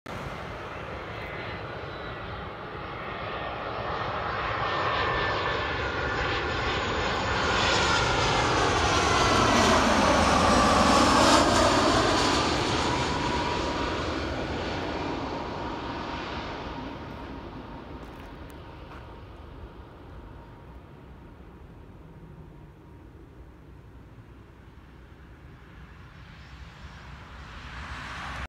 Air France Airbus A320 F-GKXJ landing Bologna airport
Air France Airbus A320 F-GKXJ

Plane age 20 years
Seat configuration Y174
Engines 2 x CFMI CFM56-5B4/P
Runway 12